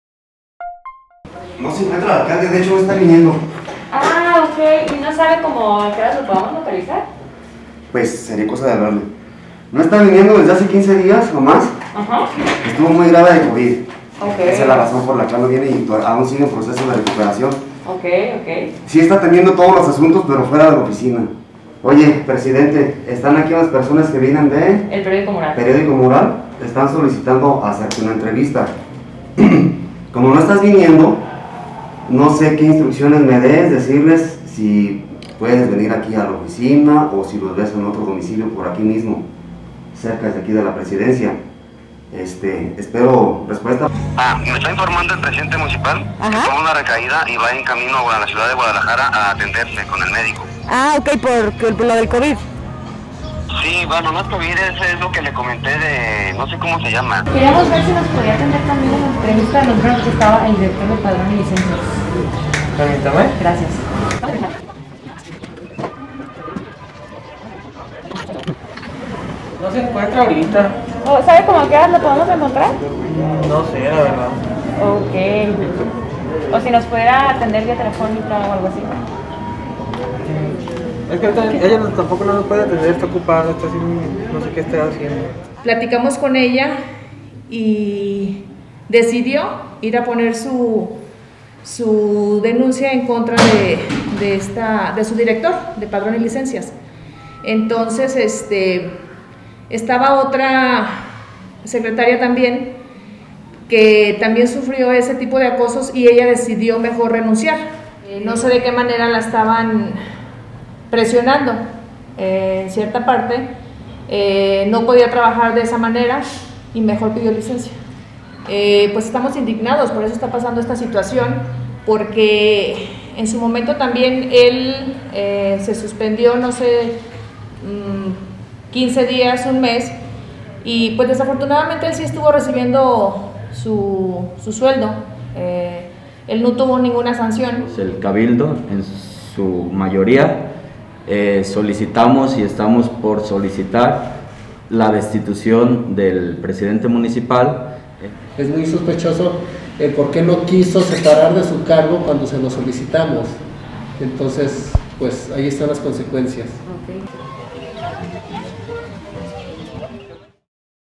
Esconden a funcionarios acosadores de Tototlán, Jalisco
Personal del Ayuntamiento de Tototlán dijo que el Alcalde Sergio Quezada no se ha presentado porque  padece secuelas de Covid-19.